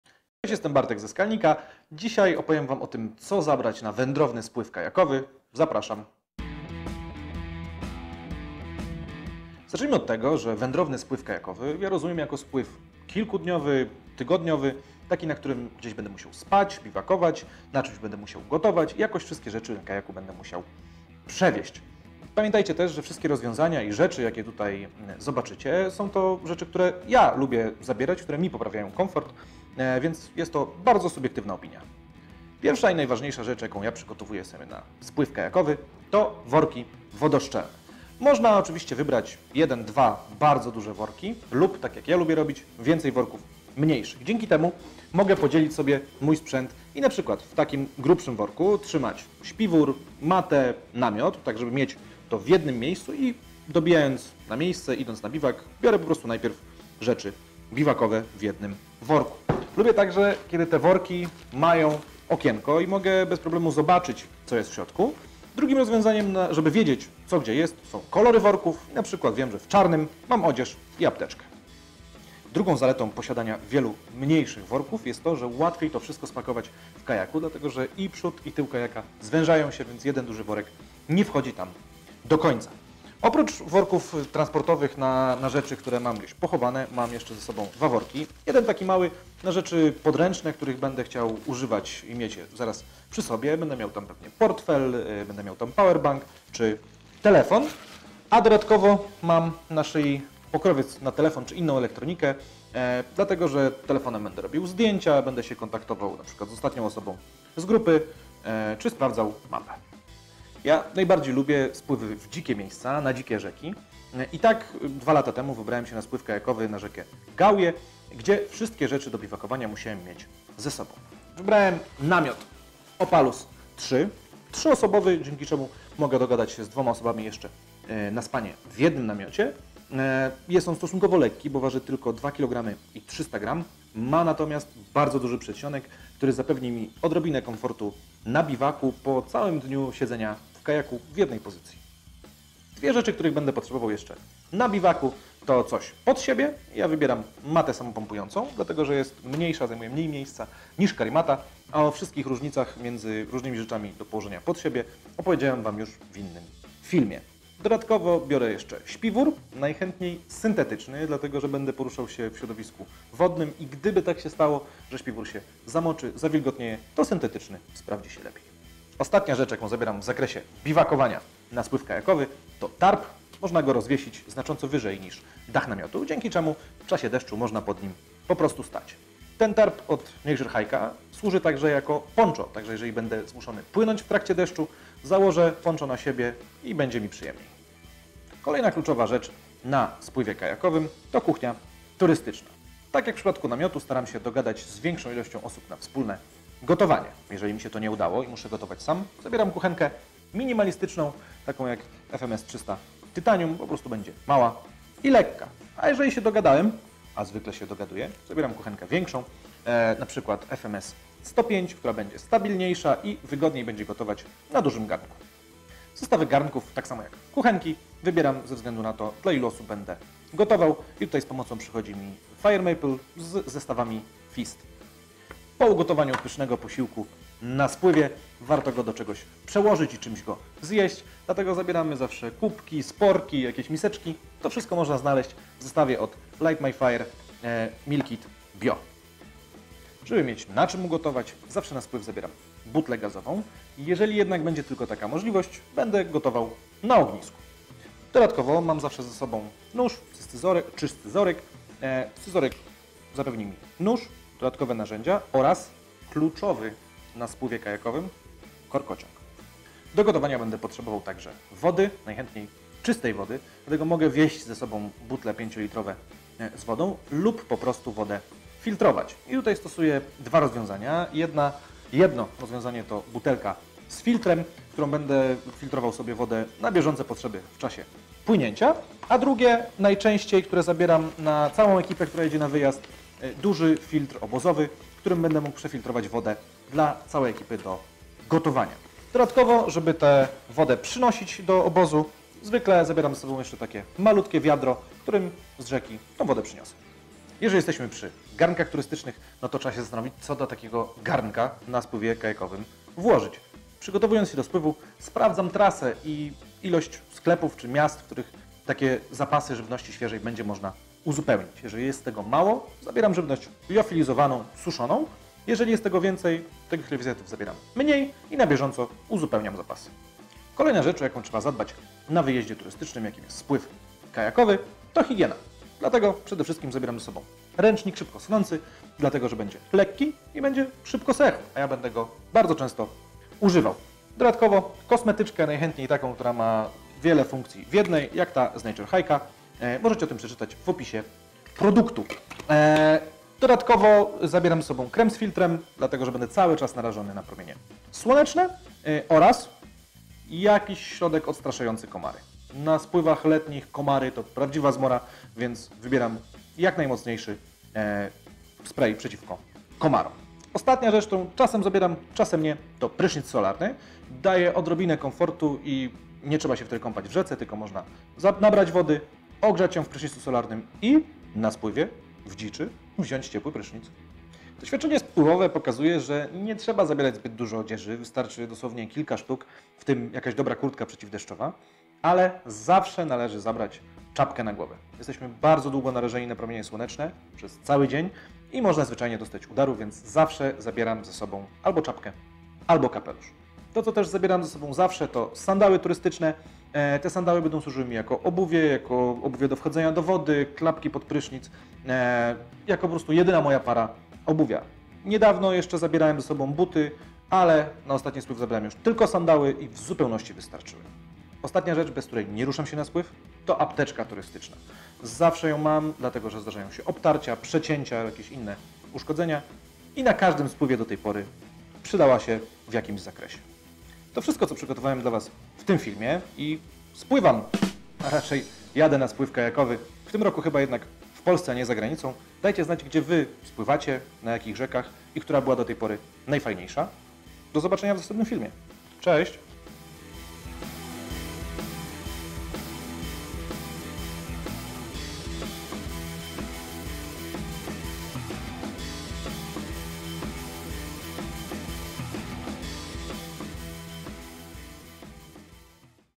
Co zabrać na spływ kajakowy - skalnik.pl
W dzisiejszym filmie zostaną przedstawione niezbędne akcesoria i sprzęt, które warto zabrać na spływ kajakowy. Bartek, nasz ekspert od turystyki kajakowej, podzieli się swoimi wskazówkami dotyczącymi przygotowań do tej przygody. Dowiesz się, jakie rzeczy mogą okazać się przydatne podczas spływu oraz jak odpowiednio przygotować się do tego rodzaju wyprawy.

Spływ kajakowy to doskonała okazja do wypoczynku na świeżym powietrzu i poznania pięknych zakątków natury. Dlatego też warto zadbać o odpowiedni sprzęt, który umożliwi nam wygodne i bezpieczne przeżycie tej przygody. Bartek pokaże, co warto zabrać na kajak, aby być przygotowanym na różne sytuacje. Sprzęt turystyczny nie tylko zapewni nam komfort podczas spływu, ale także pozwoli skupić się na cieszeniu się chwilą i otaczającym nas pięknem.

W filmie zostaną omówione również różne aspekty związane z kajakami oraz wskazówki dotyczące odpowiedniego pakowania oraz organizacji bagażu. Dzięki temu będziesz mógł lepiej planować swoje wyprawy kajakowe i uniknąć niepotrzebnego stresu związanego z zapomnieniem niezbędnych rzeczy. Spływ kajakowy to doskonały sposób na aktywny wypoczynek, dlatego warto się do niego odpowiednio przygotować i cieszyć się wyjątkowymi momentami na wodzie. Zajrzyjcie na naszą stronę po niezbędny sprzęt